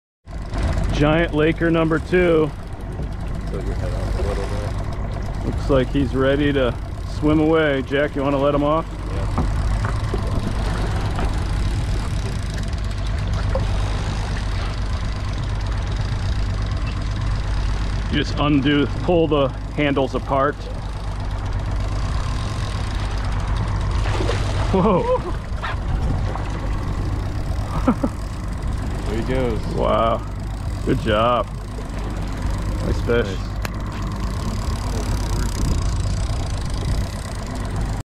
Lake Trout kept alive and released to live another day!
I've used this technique  to revive and safely release fish.  I simply tie a paracord to a Rapala lip grabber and tow the fish behind the boat until it shows signs that it's ready to go.  This fish was caught near the bottom in almost 100ft of water.  It was taken out of the water, hook removed, weighed (19lbs), picture taken, and then successfully released.  I've found this works very well with Lake Trout and King Salmon.  Steelhead not so much.